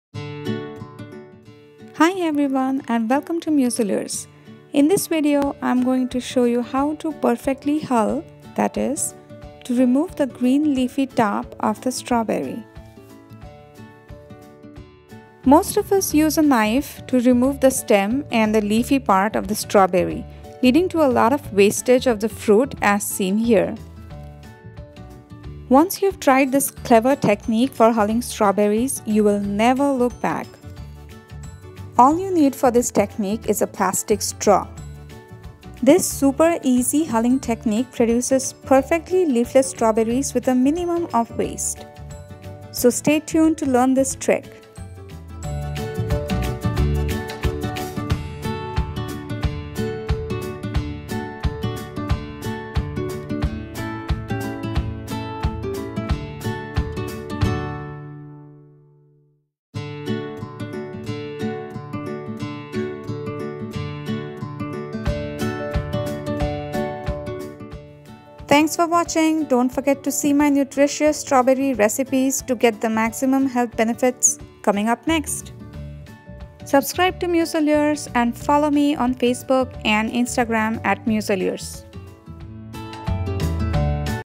How to: Hull A Strawberry Without Losing Too Much Fruit, The Quick and Easy Way!
This is the easiest way to remove the top ''stem and leaf'' part of the strawberry WITHOUT LOSING A LOT OF FRUIT, useful in prepping the strawberry in various recipes. This cooking tip will be your best find EVER! Share this hack on your social media.